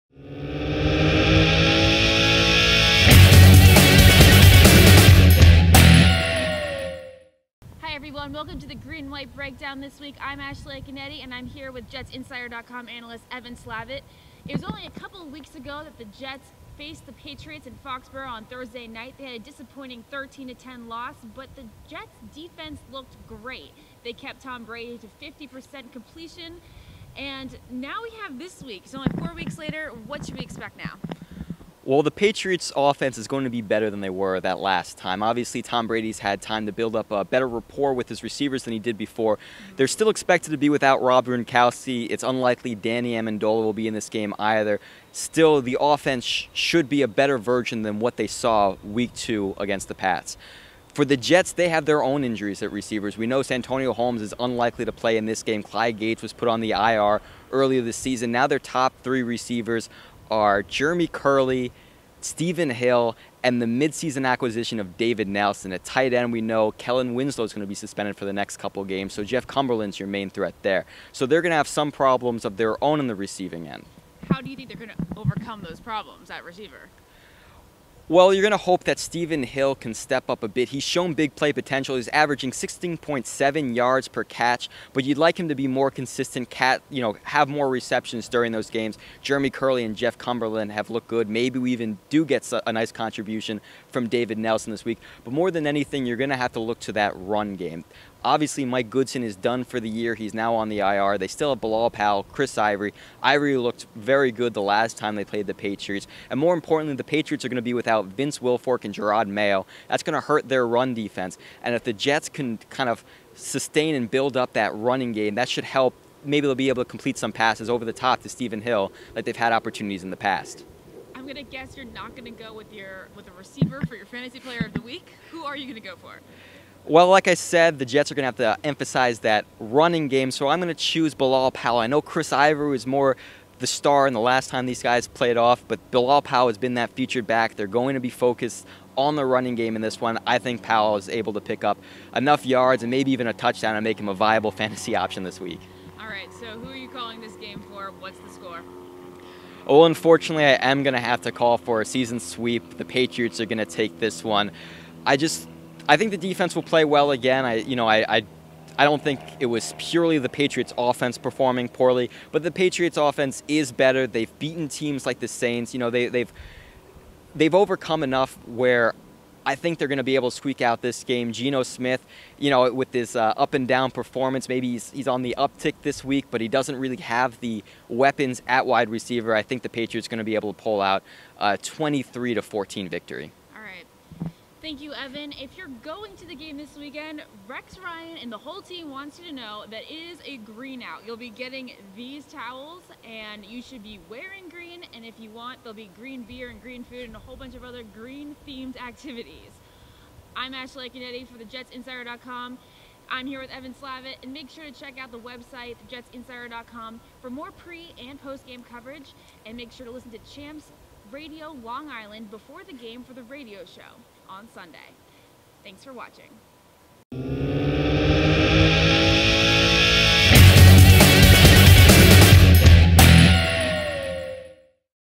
Jets v. Patriots Pre-game Breakdown
JetsInsider.com reporters Ashley Iaconetti and Evan Slavit breakdown what to expect from the Jets when the Pats come to town.